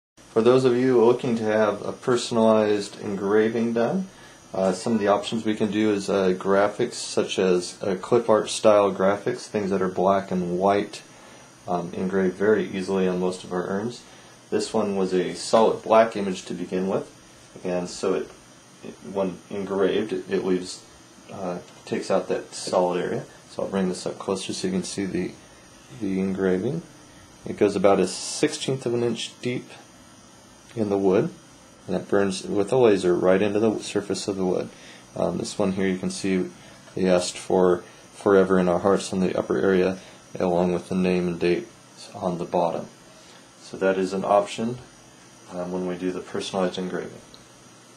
Baseball player engraved cremation urn
One of our cherry urns with a baseball player in mid swing laser engraved into the wood. Other custom engravings available.